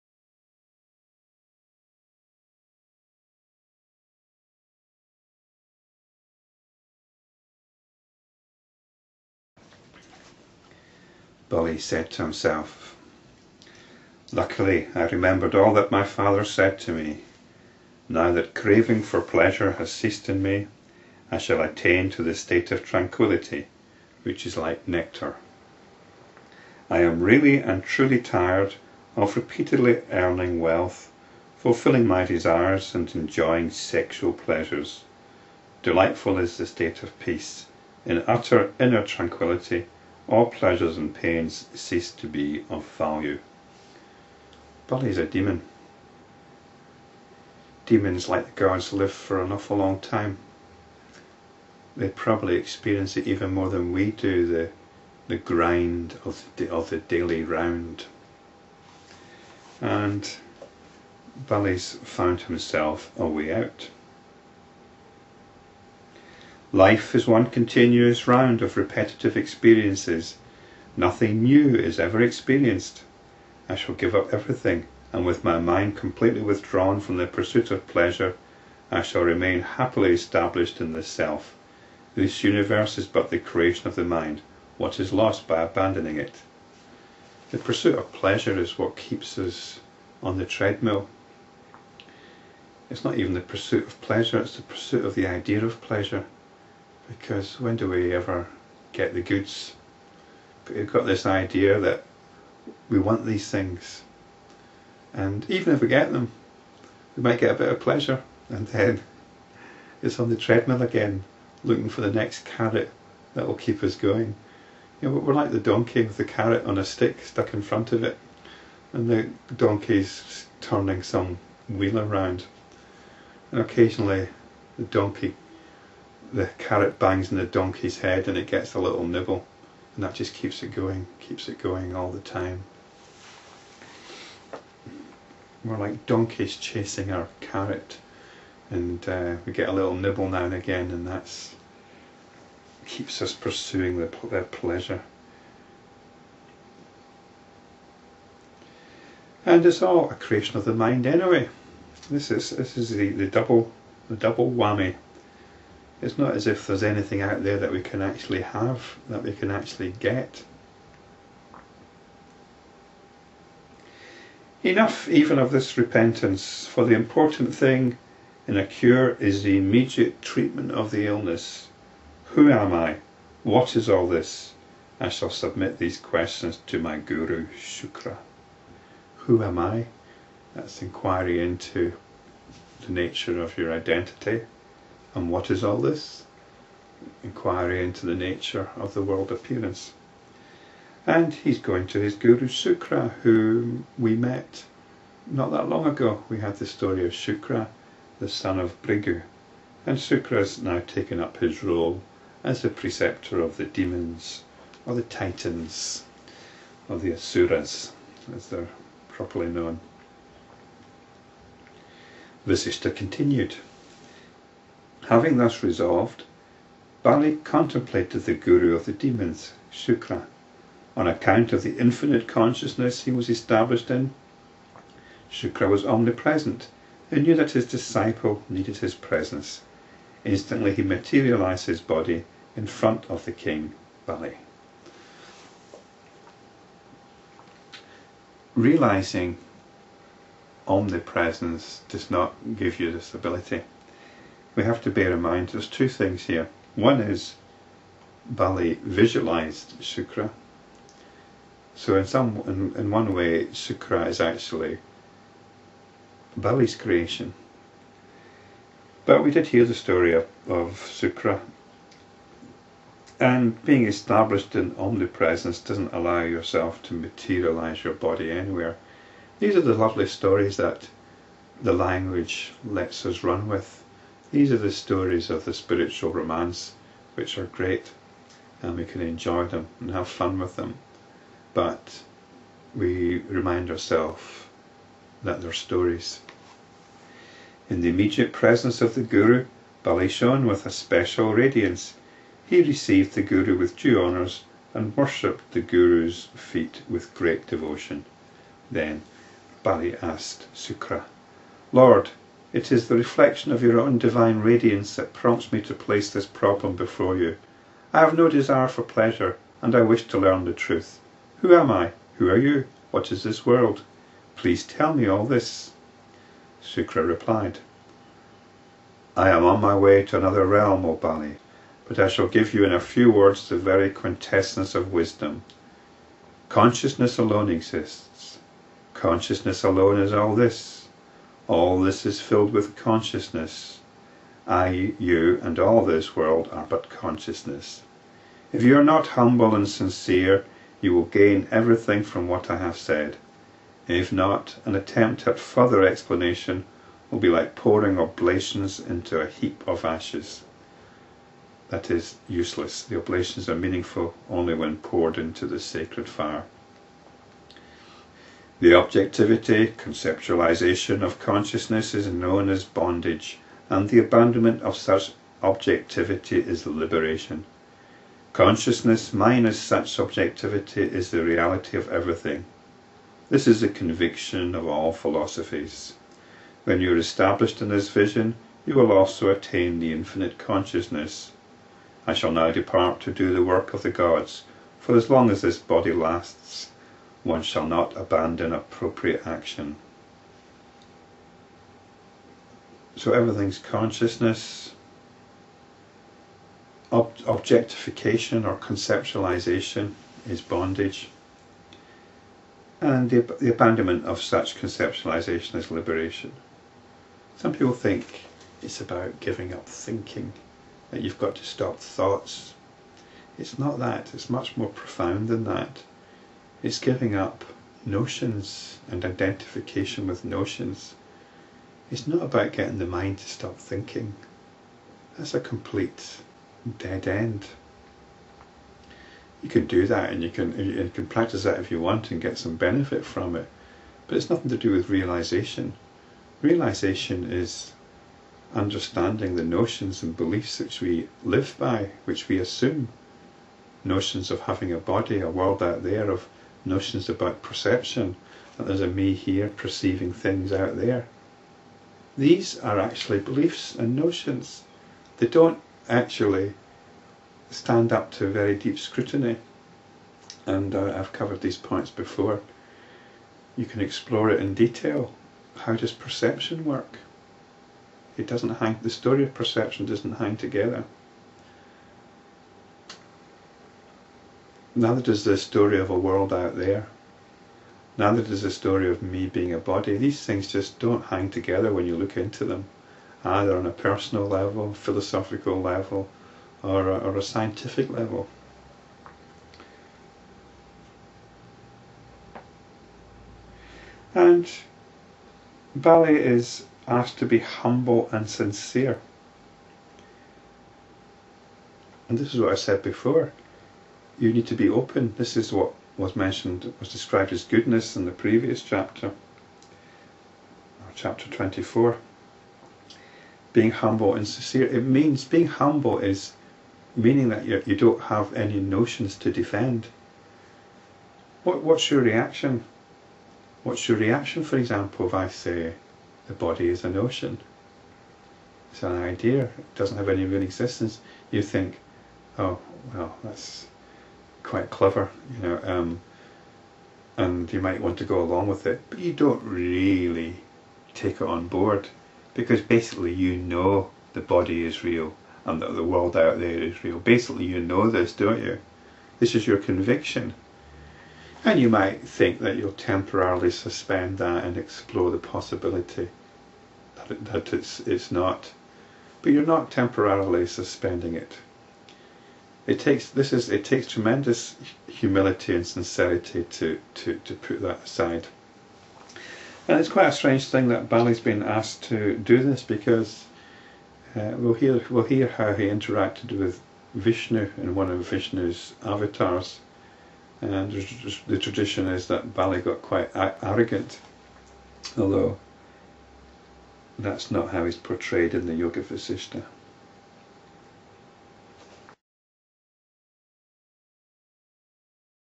0302 Section 5, Chapters 25,26 Yoga Vasishta
Bali has given up the pursuit of wealth and sex.  He's found the way out in the tranquil equanimity of the self.

The pursuit of the idea of pleasure keeps us on the treadmill.  The occasional nibble keeps us at it.

How can we actually have anything?

"Who am I?"  "What is all this?"  Bali turns to his guru Sukra through visualisation.  Being established in omnipresence does not give us supernormal abilities!

Consciousness alone exists and is all this.  Humility and sincerity is necessary to appreciate this.  Conceptualisation leads to bondage.  When consciousness frees itself from its conceptualising tendencies then this is liberation.

Liberation is not about giving up thinking.  It's really about our bondage to notions. Our notions are the stories that we live by.  We don't really want to accept that they are stories...

Text Yoga Vasistha Book 1, August 20